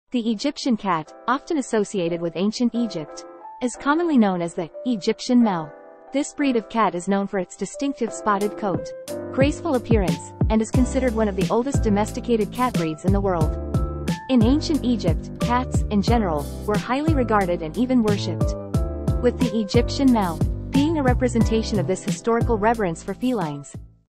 What is the Egyptian cat called?
The Egyptian cat, often associated with ancient Egypt, is commonly known as the "Egyptian Mau." This breed of cat is known for its distinctive spotted coat, graceful appearance, and is considered one of the oldest domesticated cat breeds in the world. In ancient Egypt, cats, in general, were highly regarded and even worshiped, with the Egyptian Mau being a representation of this historical reverence for felines.